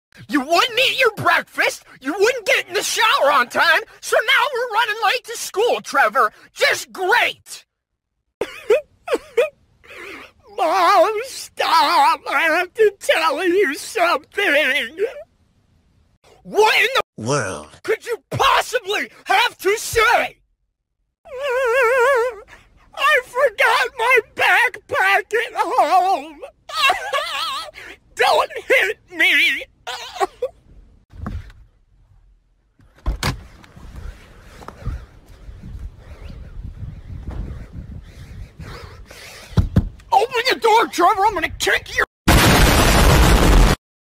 TSC Snaps At Red | AvM (Fan-Made)
I got quite lazy on this, sorry.

• New watermark
• New text style (just for this video!)

Characters by Alan Becker
Audio by @thetrevorabney on TikTok
Inspiration from ​​⁠​⁠@HendiTheHen
Programs used: ibisPaint (drawing), CapCut (tweening)

You may use this video for complications/reaction videos, etc. Please do not crop my watermark off (or you can give credits at the end).

Remember to stay safe and healthy and have a nice day.